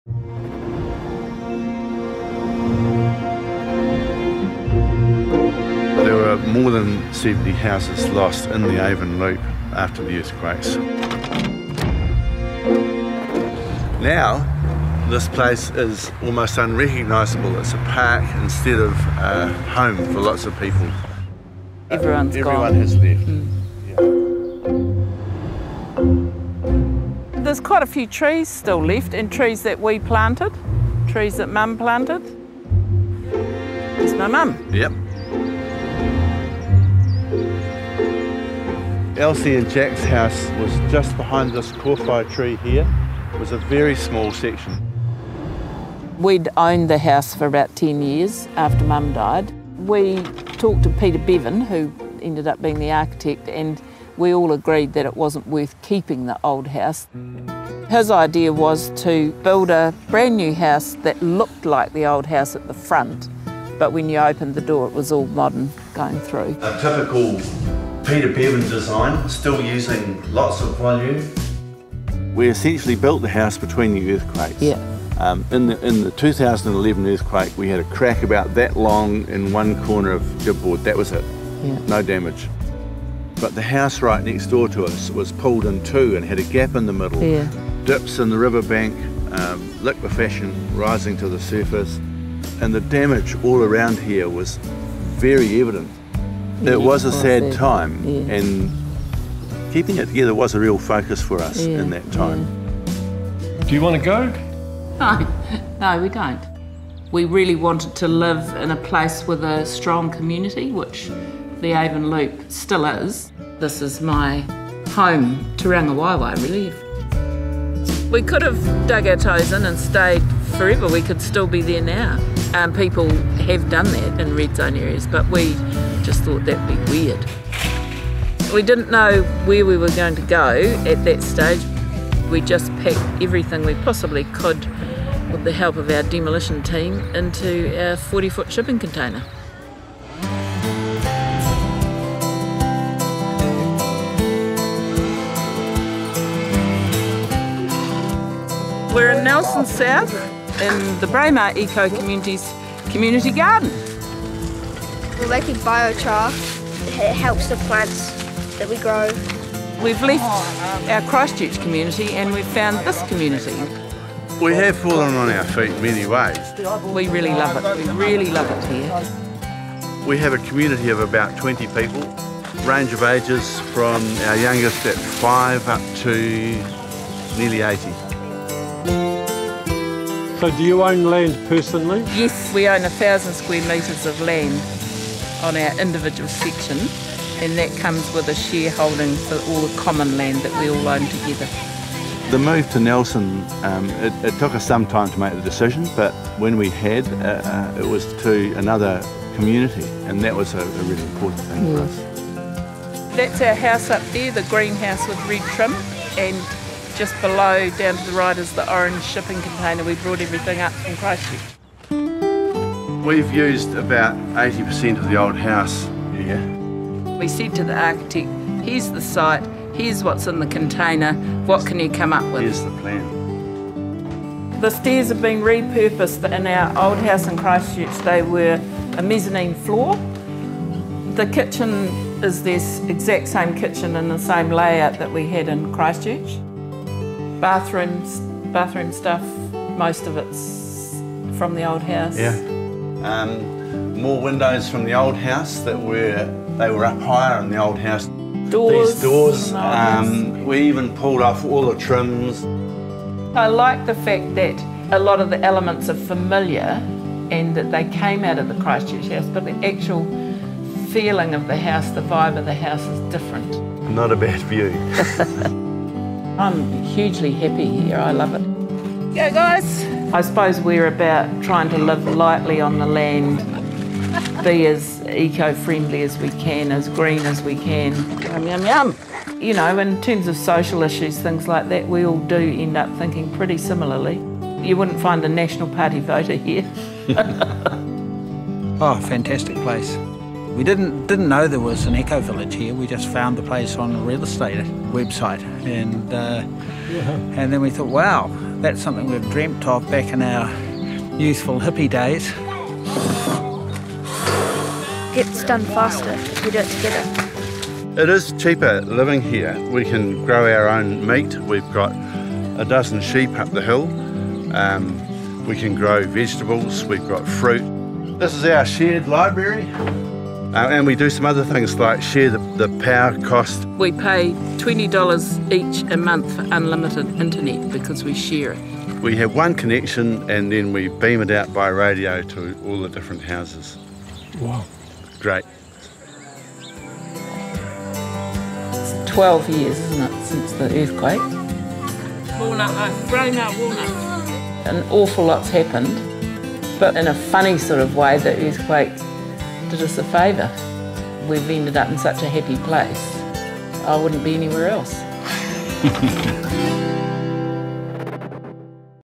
Frank Film: Finding a new community | nzherald.co.nz
Mike Moss and Alison Locke have returned to Christchurch’s Avon Loop Red Zone, soaking up memories of their former home.